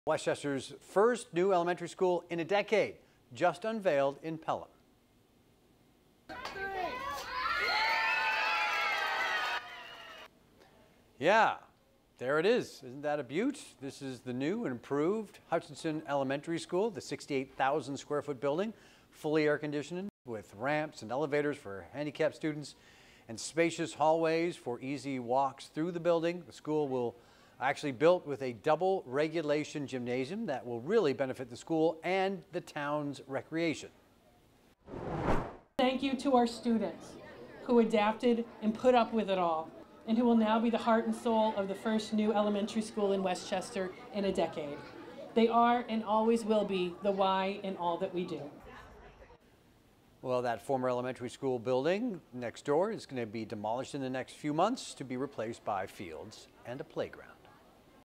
New and improved Hutchinson Elementary School unveiled in Pelham
The new and improved Hutchinson Elementary School is a 68,000-square-foot building that is fully air conditioned with ramps and elevators for students with disabilities. It also has spacious hallways for easy walks through the building.